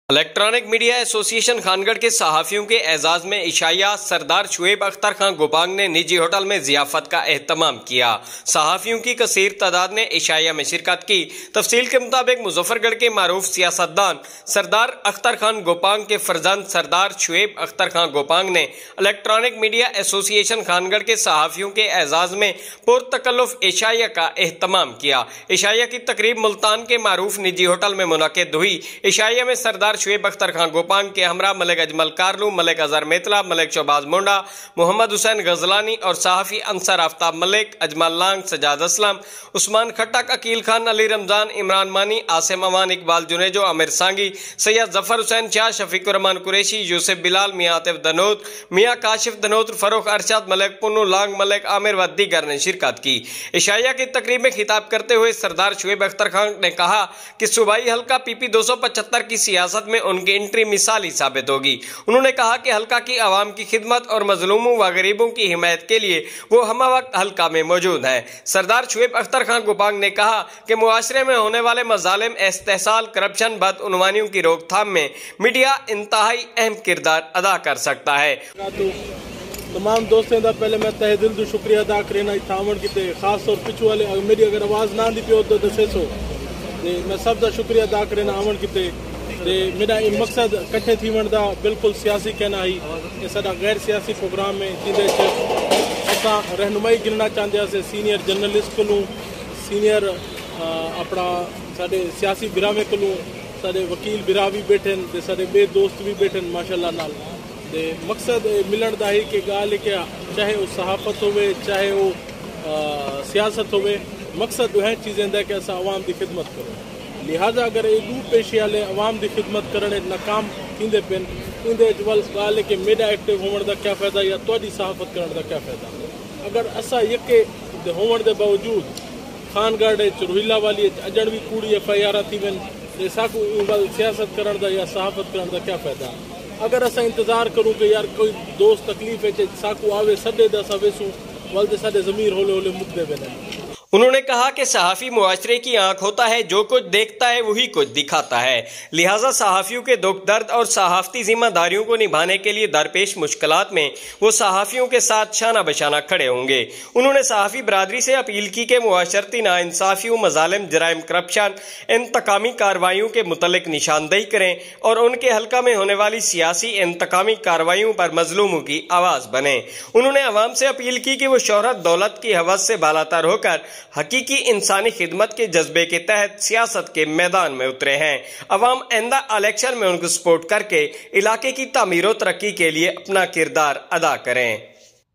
Dinner Journalists||Sardar Shuaib Akhtar Khan Gopang||Electranic Media Association Khangarh
Sardar Shuaib Akhtar Khan Gopang Gave Dinner to journalists of Electranic Media Association Khangarh Muzaffargarh on hotel